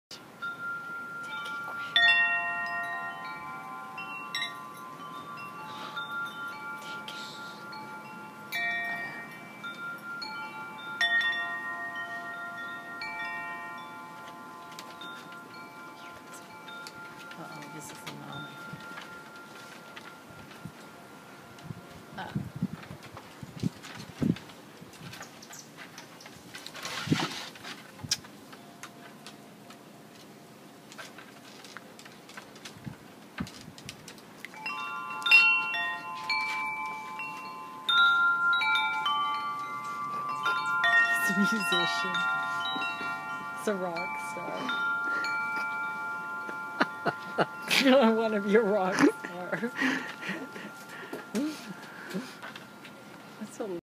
Musical baby raccoon
Of the 6 baby raccoons in our 'hood, one seems to be musically inclined and loves to play our wind chime.